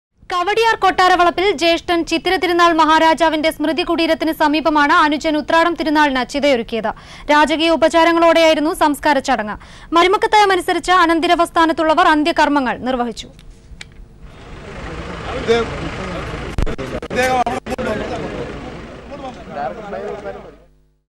Uthradom Thirunal Marthanda Varma cremated with state honours
Uthradom Thirunal Marthanda Varma (91), head of the erstwhile Travancore royal family, passed away at a private hospital in Thiruvananthapuram on Monday morning.